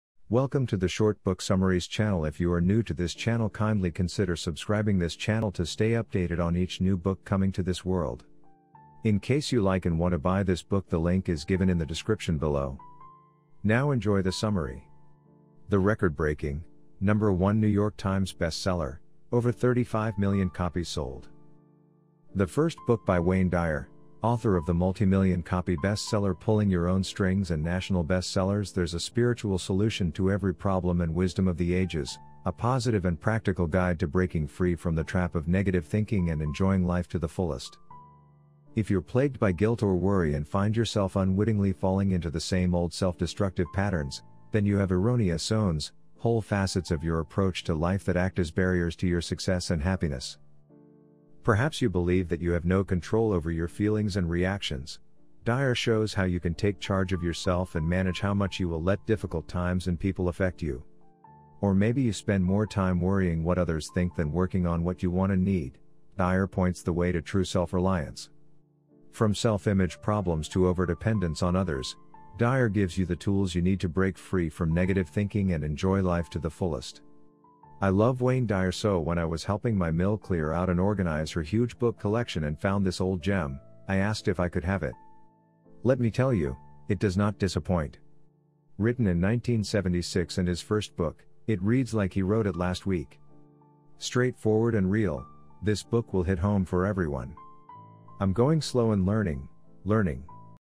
Short Book Summary of Your Erroneous Zones by Wayne W Dyer
THE RECORD-BREAKING, #1 NEW YORK TIMES BESTSELLER – OVER 35 MILLION COPIES SOLD

The first book by Wayne Dyer, author of the multimillion-copy bestseller Pulling Your Own Strings and national bestsellers There’s a Spiritual Solution to Every Problem and Wisdom of the Ages, a positive and practical guide to breaking free from the trap of negative thinking and enjoying life to the fullest.

If you're plagued by guilt or worry and find yourself unwittingly falling into the same old self-destructive patterns, then you have "erroneous zones" – whole facets of your approach to life that act as barriers to your success and happiness.

Perhaps you believe that you have no control over your feelings and reactions – Dyer shows how you can take charge of yourself and manage how much you will let difficult times and people affect you. Or maybe you spend more time worrying what others think than working on what you want and need – Dyer points the way to true self-reliance. From self-image problems to over-dependence on others, Dyer gives you the tools you need to break free from negative thinking and enjoy life to the fullest.
I love Wayne Dyer so when I was helping my MIL clear out and organize her huge book collection and found this old gem, I asked if I could have it. Let me tell you, it does not disappoint. Written in 1976 and his first book, it reads like he wrote it last week. Straight forward and real, this book will hit home for everyone! I'm going slow and learning, learning.